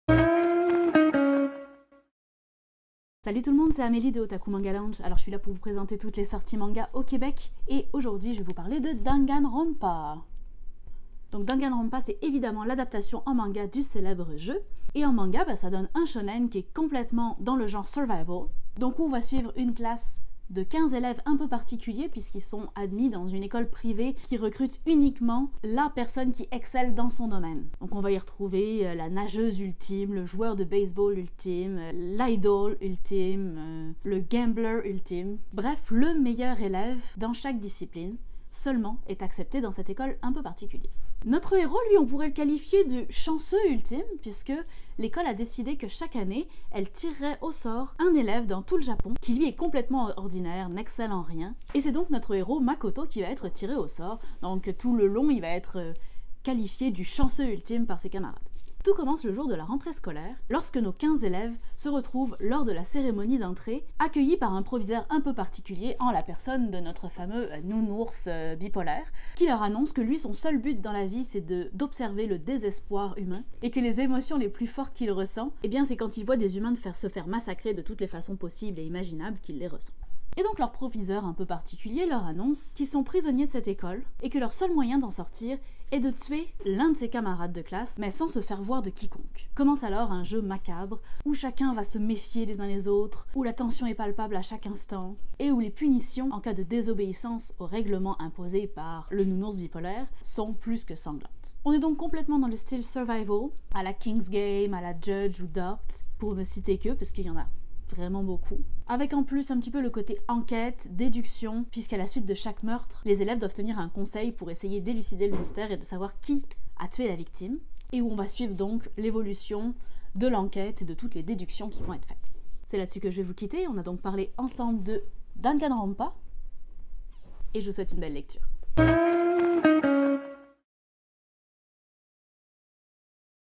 Mangathèque O-Taku: Danganronpa - shonen survival #1
Enfin une adaptation manga en français pour le célèbre jeu Danganronpa. Et ça donne de l'explosif !